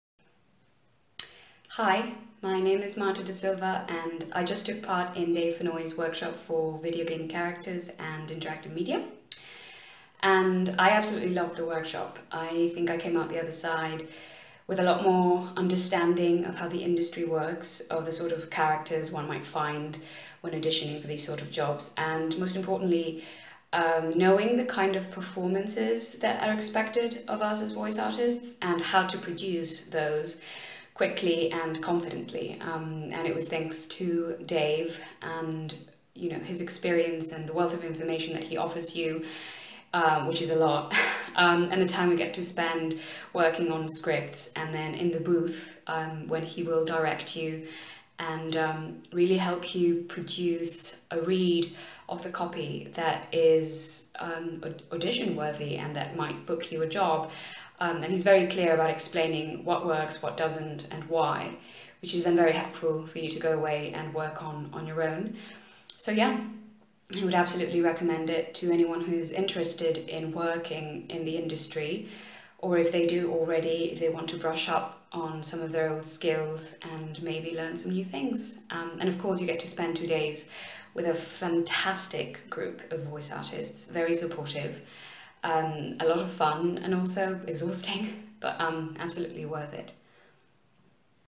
Marta Da Silver talking about her experience on the VON Dave Fennoy workshop
This is a video of one of The VoiceOver Network members talking about her experience on our 'Character development for video games' workshop with Dave Fennoy. Marta took part in both workshops that Dave taught in London in 2016. Here she tells us why she thinks it's so important.

Dave is coming back to London to teach another workshop exclusively for The VoiceOver Network on 28th, 29th and 30th April 2017.